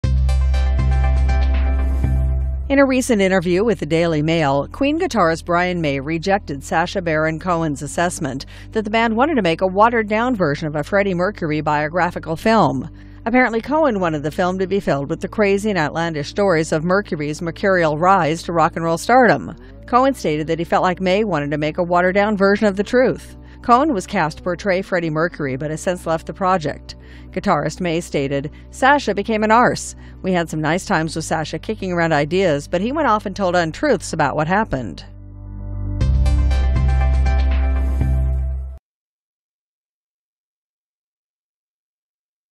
Brian May Of Queen Said Sacha Baron Cohen Is An Arse
In a recent interview with The Daily Mail, Queen guitarist Brian May rejected Sacha Baron Cohen's assessment that the band wanted to make a watered down version of a Freddy Mercury biographical film. Apparently Cohen wanted the film to be filled with the crazy and outlandish stories of Mercury's mercurial rise to rock and roll superstardom. Cohen stated he felt like Queen guitarist Brian May wanted to make a watered down version of the truth. Cohen was cast to portray Freddie Mercury but has since left the project. Guitarist, May stated, ""Sacha became an arse. We had some nice times with Sacha kicking around ideas, but he went off and told untruths about what happened."